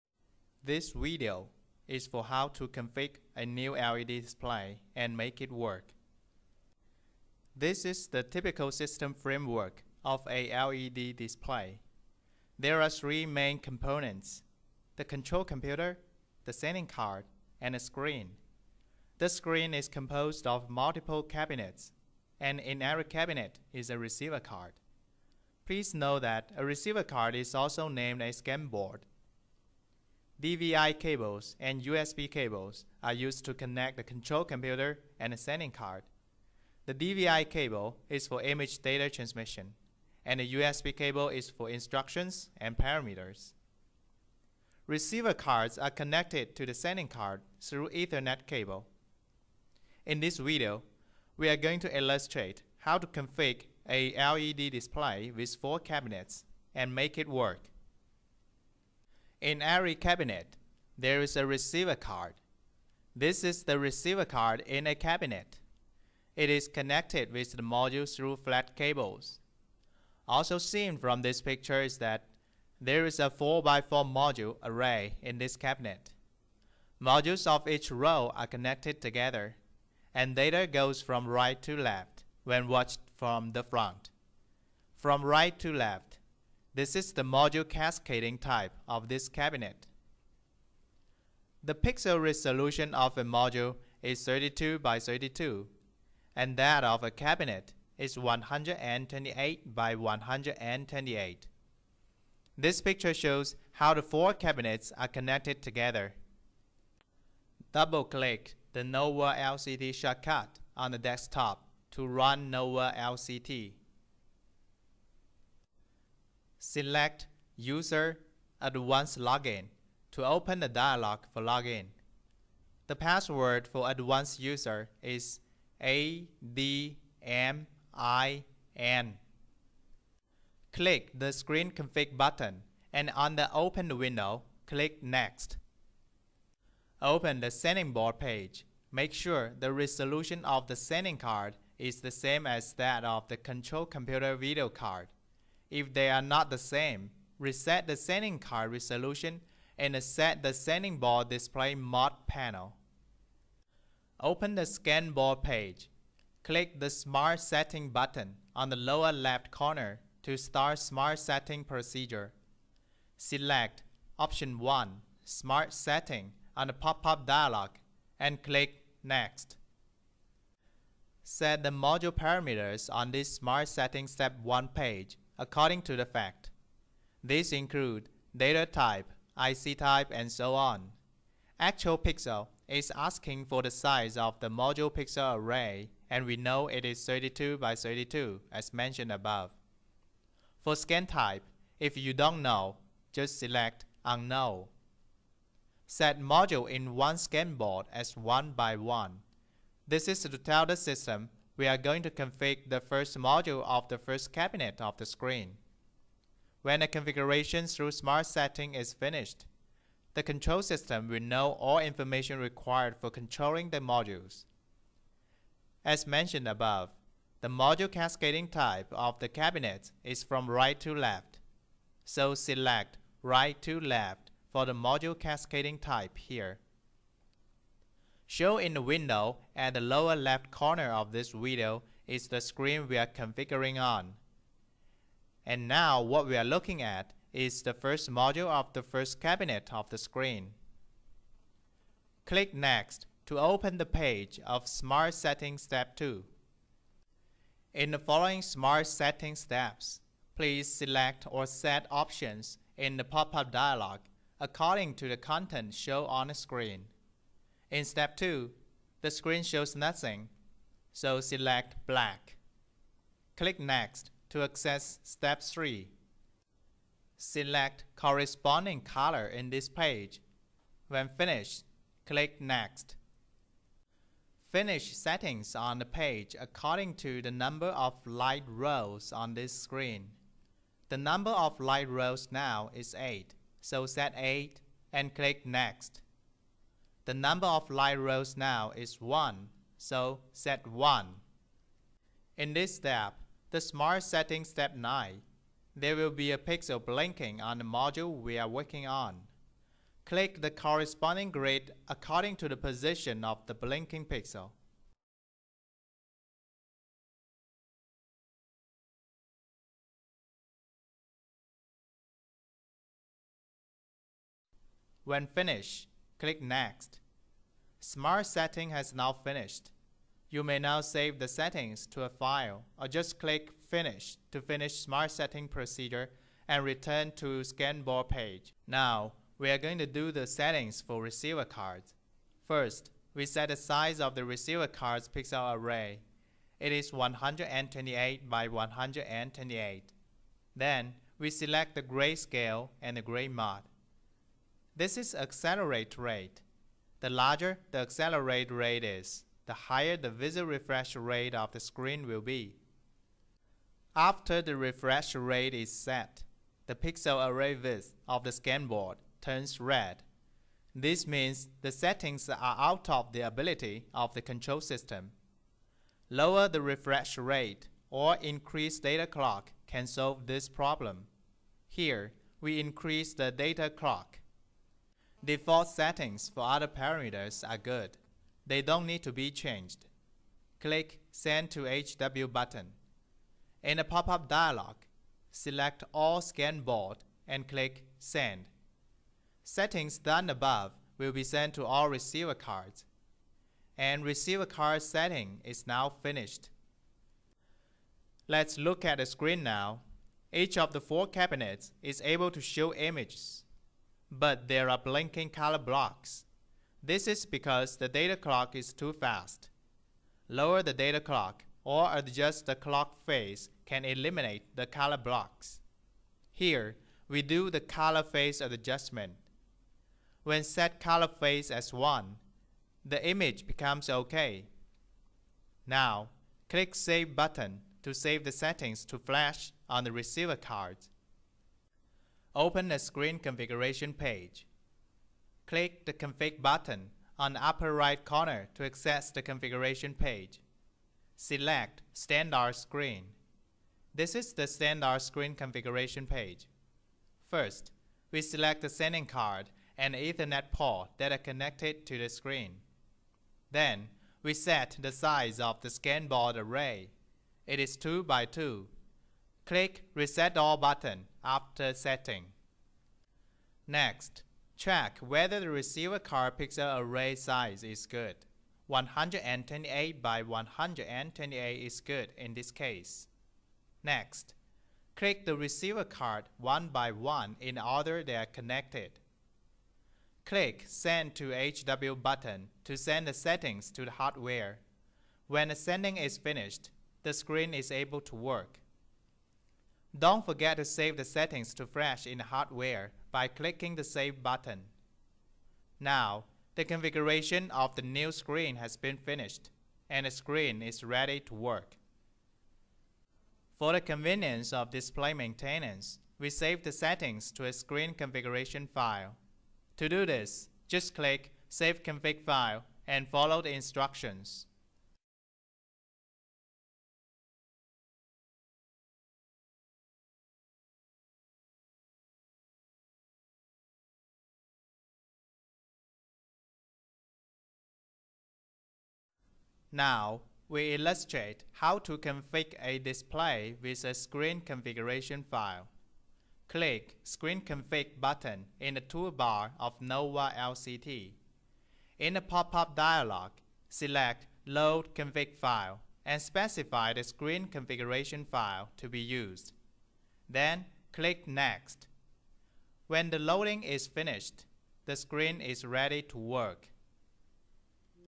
Tutorial of LED display control software Novastar-How to configure LED screen
We will show you how to use NOVA software to configure led display.